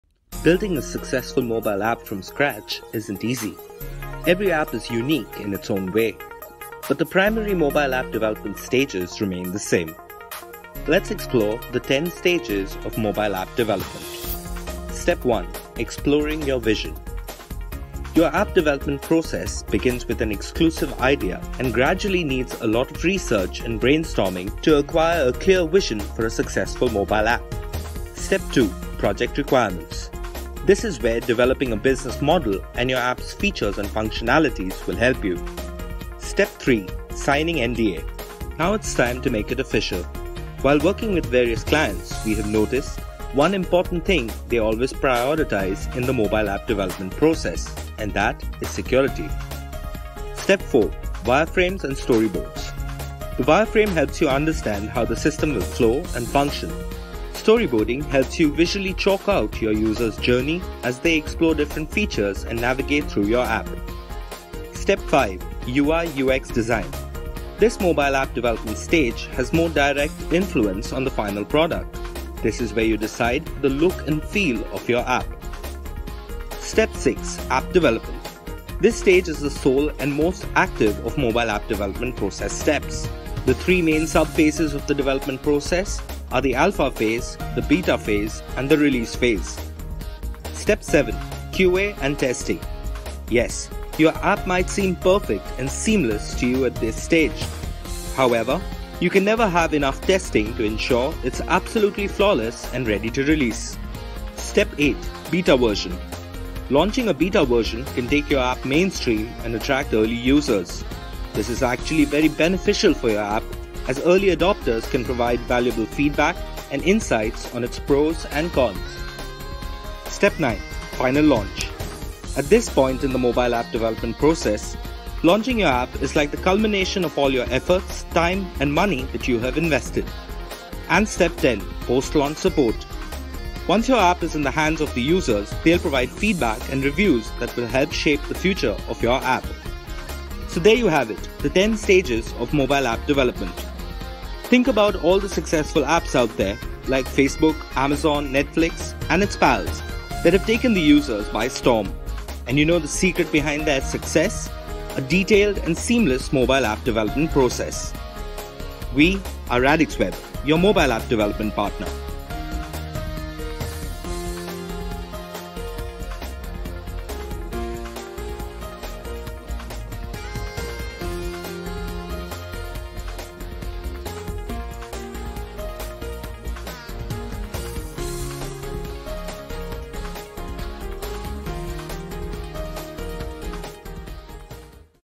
10 stages of effective Mobile Application Development
Your app development process begins with an exclusive idea and gradually needs a lot of research and brainstorming to acquire a clear vision for a successful mobile app.
Building a successful mobile app from scratch isn’t easy. Every app is unique in its own way, but the primary mobile app development stages remain the same.
Let’s explore the 10 stages of mobile app development in our exclusive video.